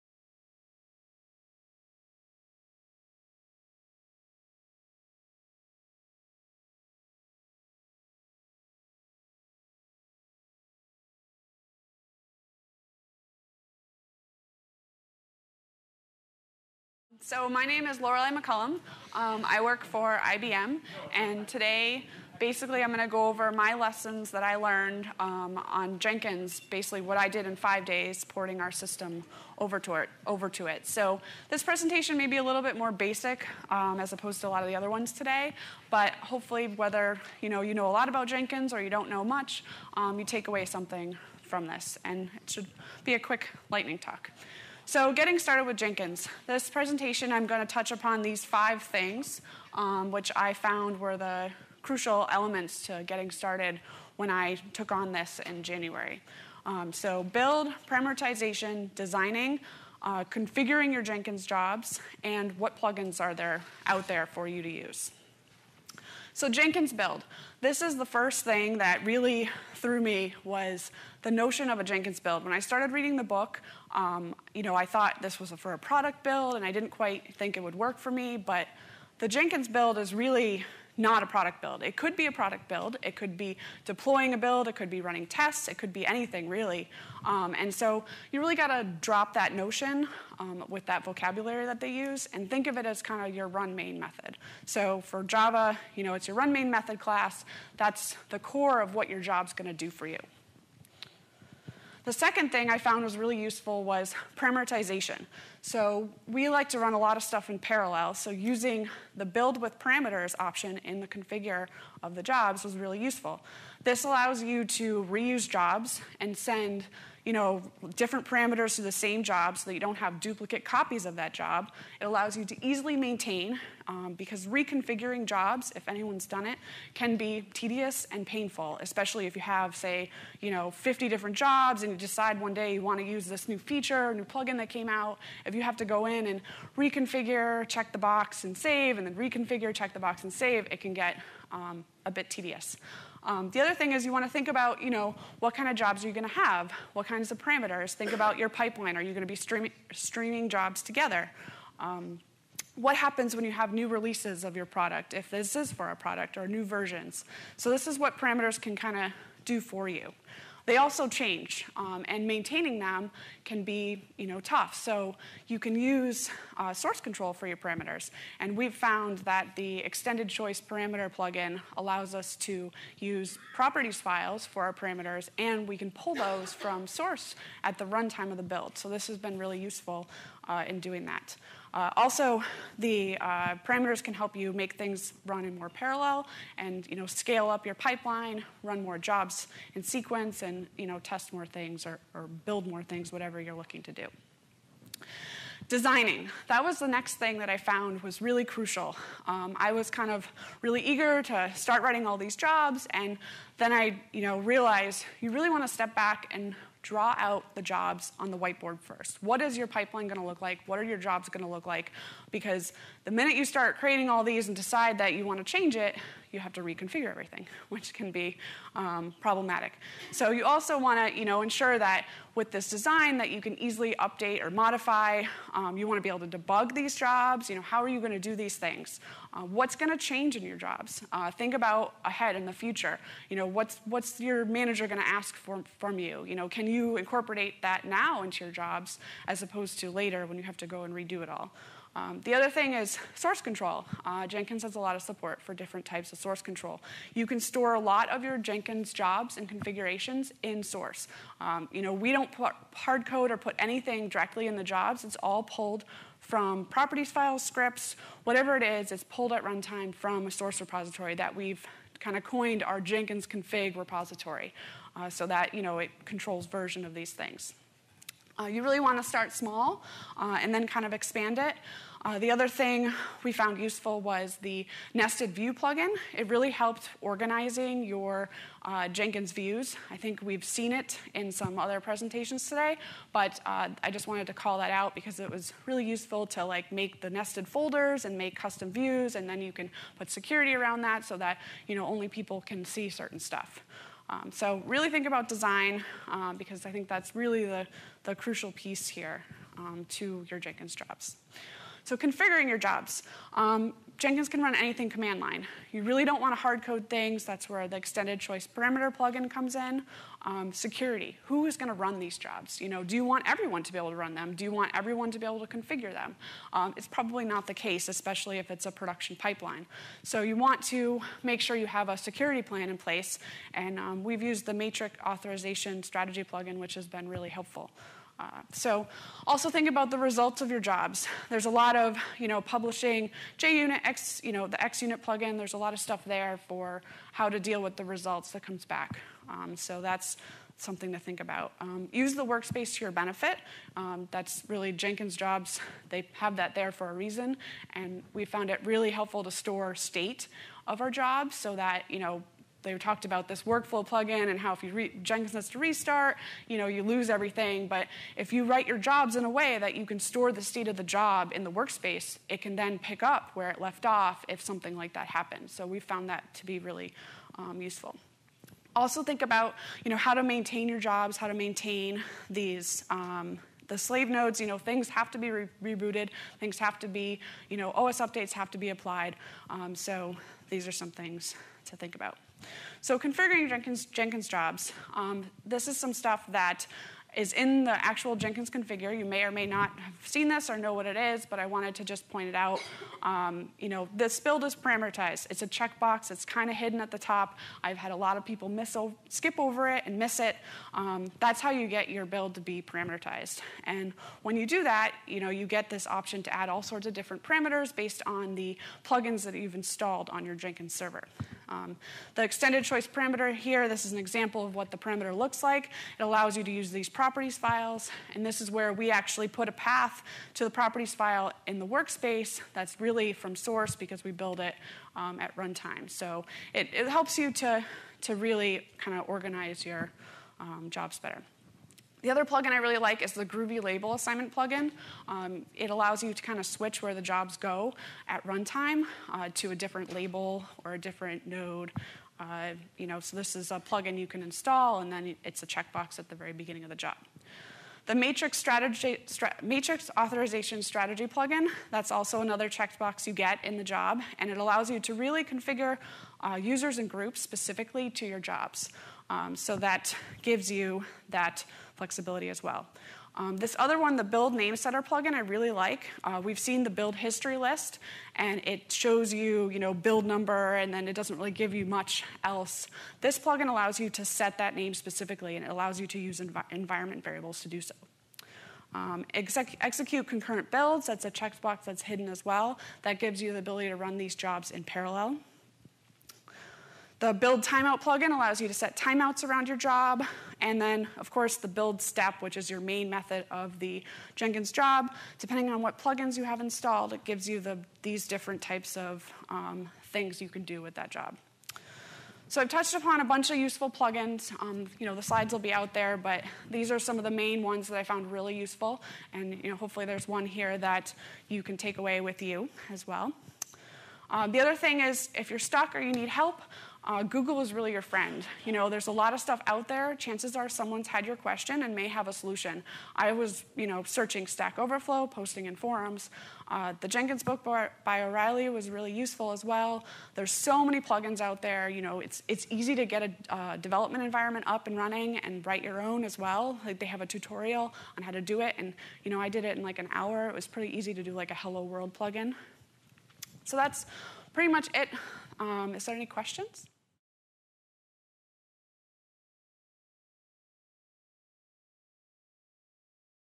2014 JUC Boston - Zero to Jenkins in Five Days
By Lorelei McCollum

Lessons learned from running Jenkins for five days with no prior knowledge. Jenkins allows for many different configurations and plugins for building a test automation pipeline, however design, source control of Jenkins artifacts, parametrization to eliminate duplication are key elements to ensure scalability is executed through a new pipeline upon product build availability. These best practices will get you there in less than five days.